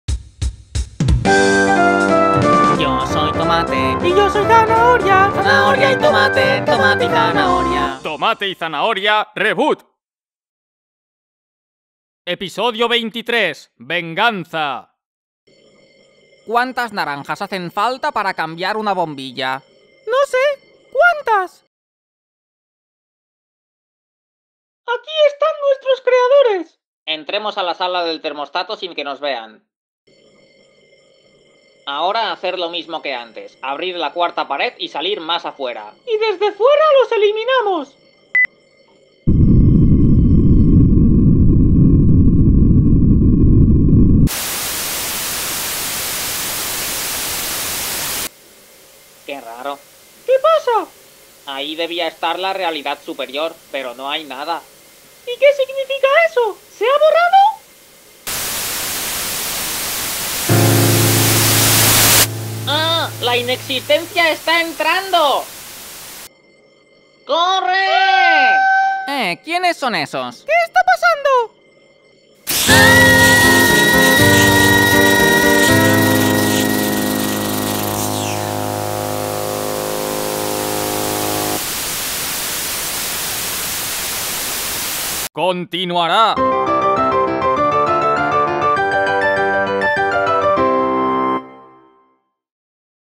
T&Z Reboot: 1x23 - VENGANZA
Los del 2do reboot han llegado al universo del 1er reboot, y se disponen a ejecutar su venganza...

Tomate & Zanahoria REBOOT - Temporada 1 Episodio #23: "VENGANZA"